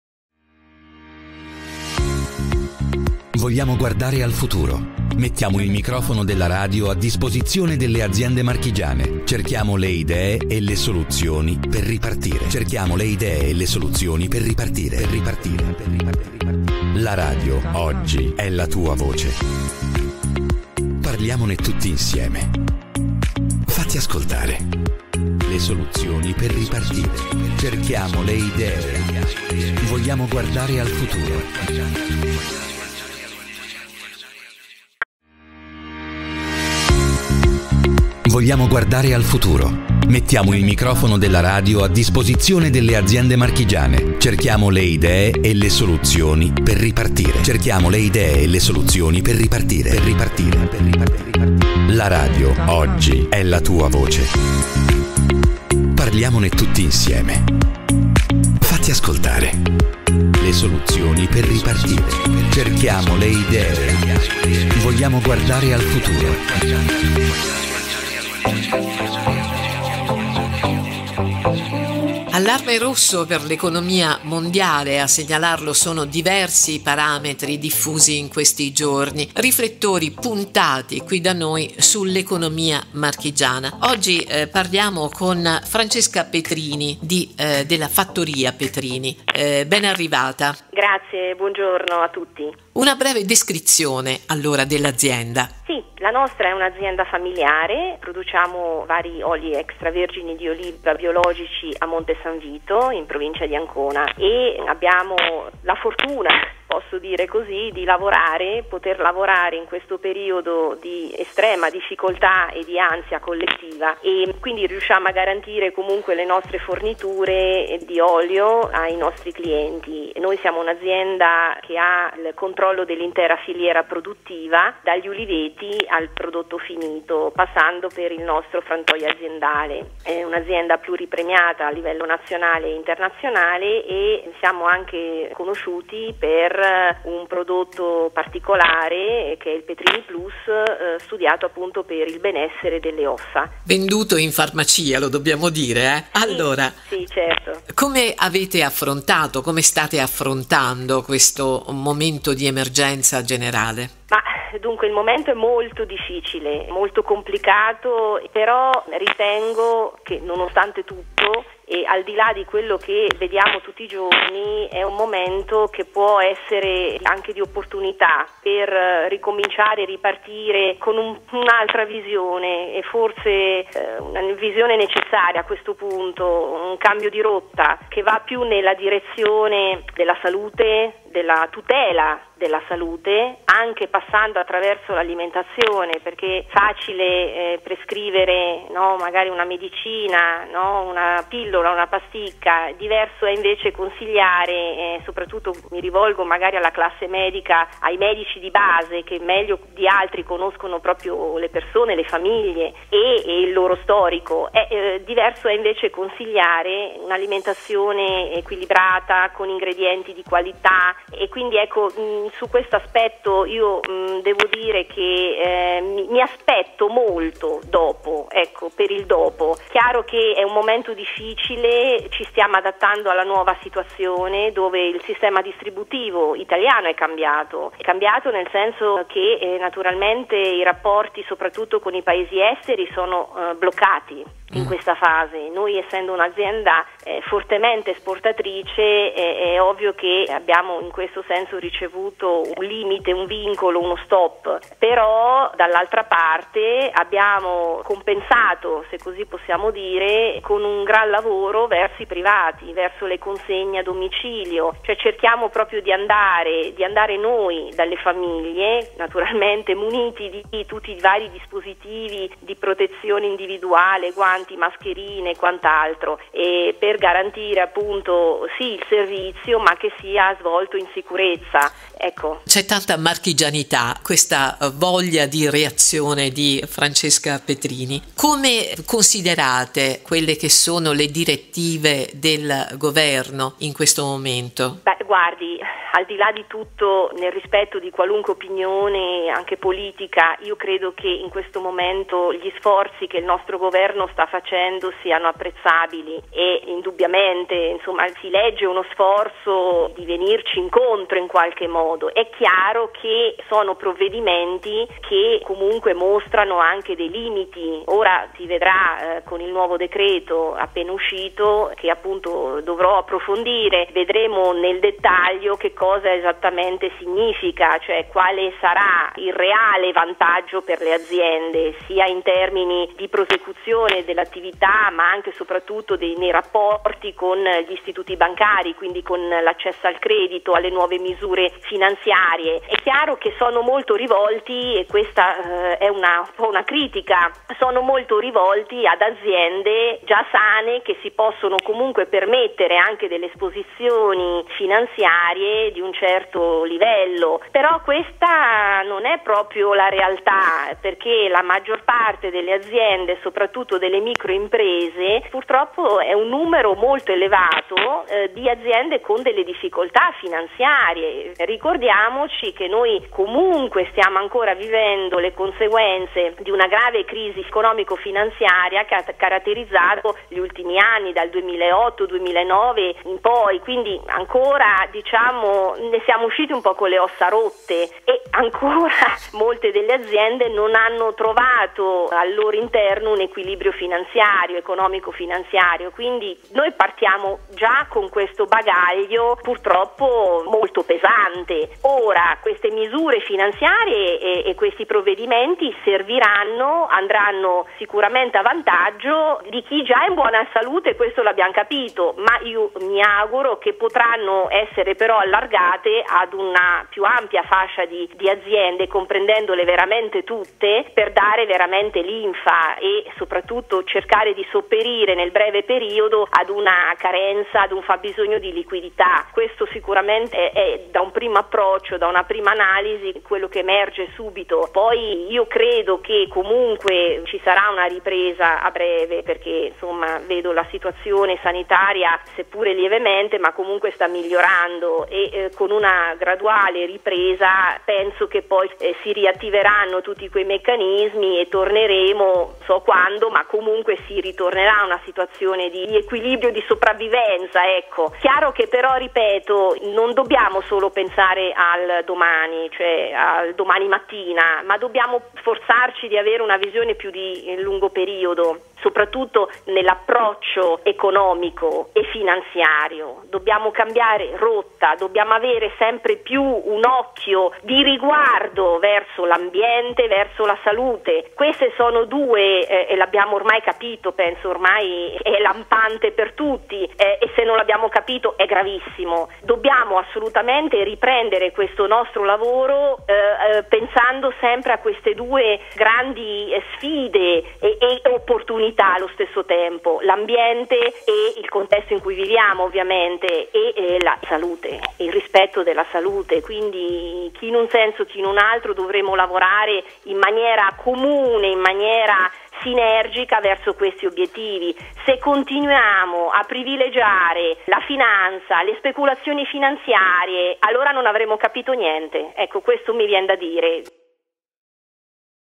Intervista aFrancesca Petrini
Francesca Petrini sottolinea l'importanza di rivolgersi ad un consumo di qualità caratteristica fondamentale per vivere una vita più sana.